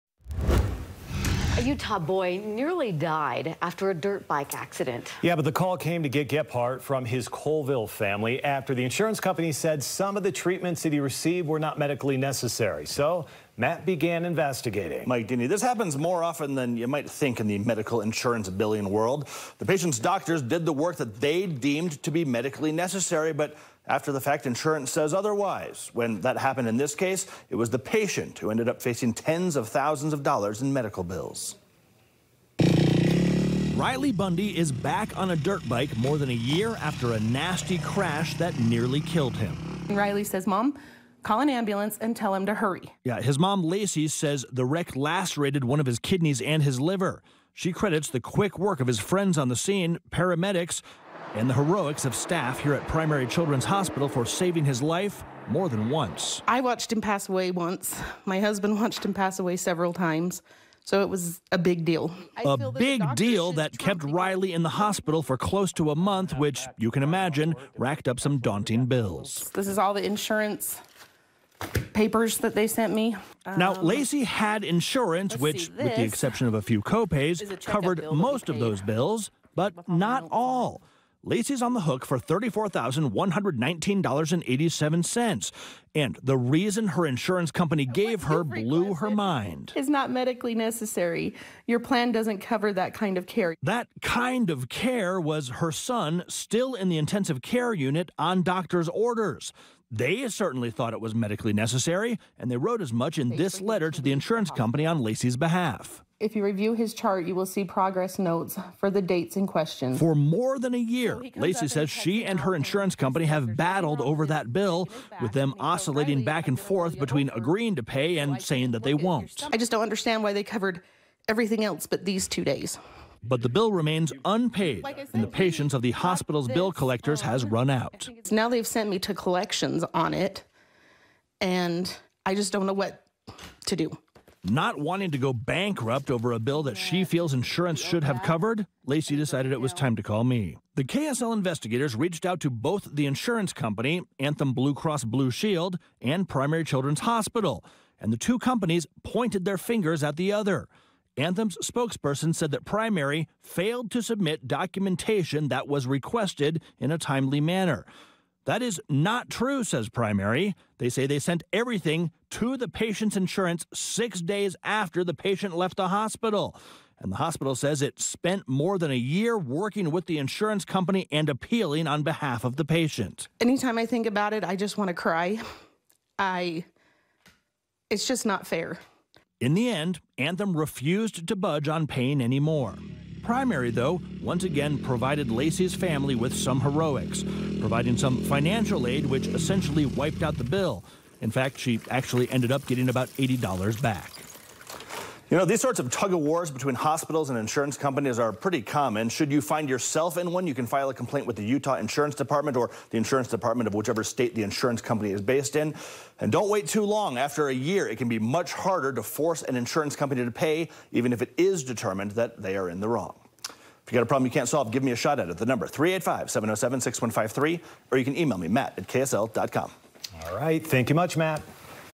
‘Medically necessary’: Conflict between hospital and insurance leaves Utah family owing over $30K
A Coalville family says an emergency surgery helped save their son’s life. Yet, their insurance company denied coverage saying the procedure was not medically necessary. Left owing tens-of-thousands of dollars, the family decided to Get Gephardt.